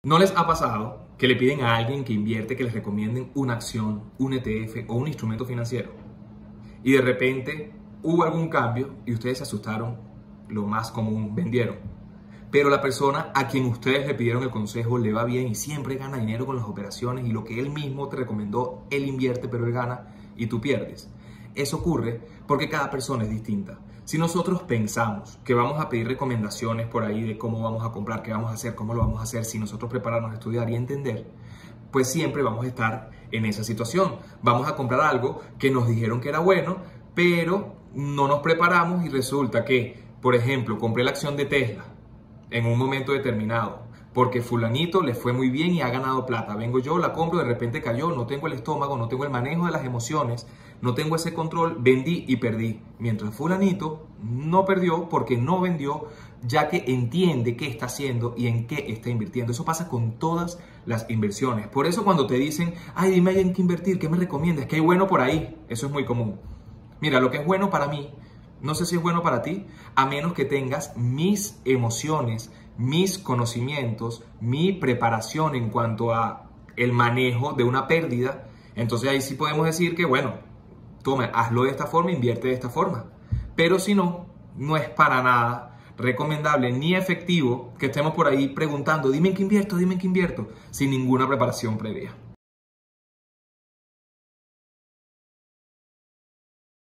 ¿POR QUÉ NO GANAS DINERO CON LA RECOMENDACIÓN QUE TE DIERON?
En este video explico por qué una inversión puede ser buena para una persona y mala para otra.